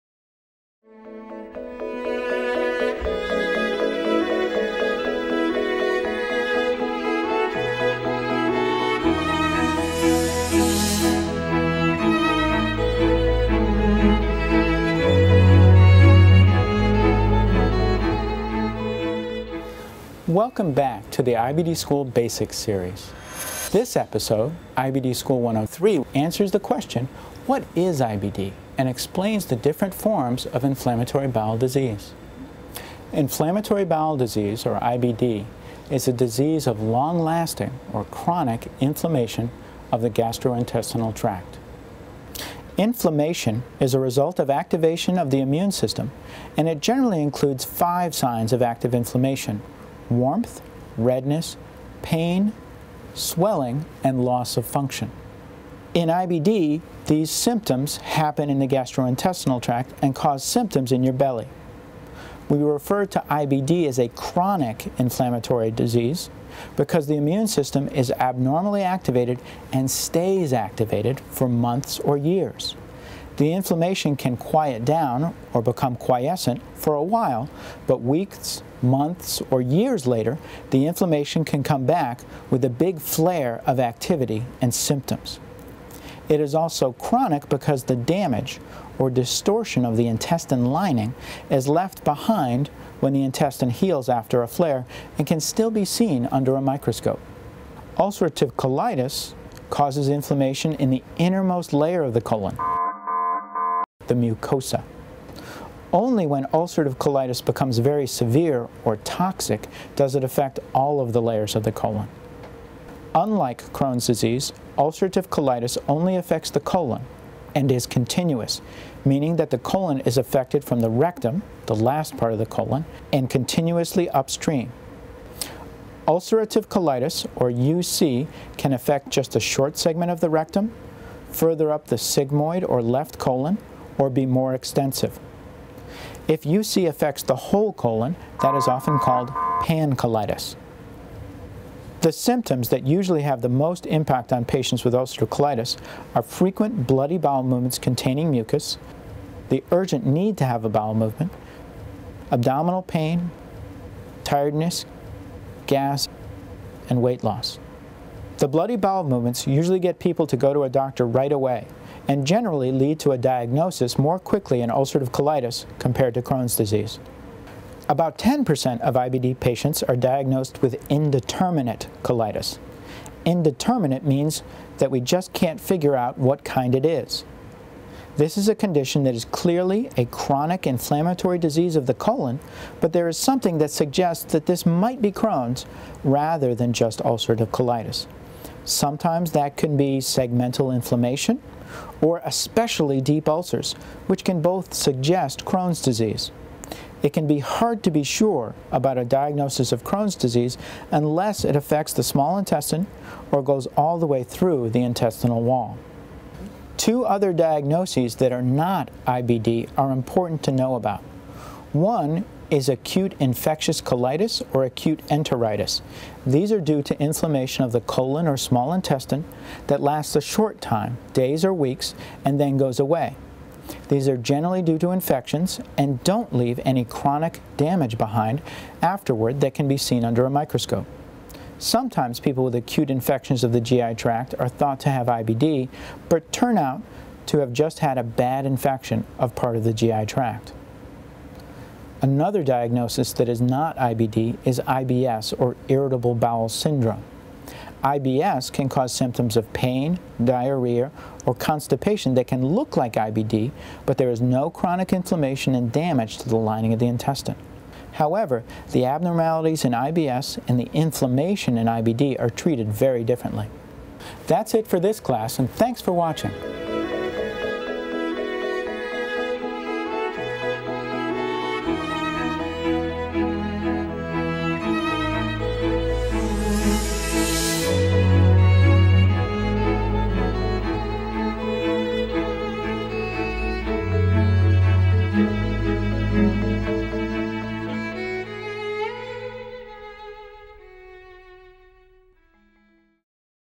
IBD School 103 - What is ulcerative colitis?
This is the third video in the IBD School Basics Video Series, which aims to educate IBD patients, their friends, and families of people with IBD about Crohn's Disease and ulcerative colitis. This video explains the basics of ulcerative colitis. Please add comments, make suggestions for improvements, and suggest topics for future videos. Thanks!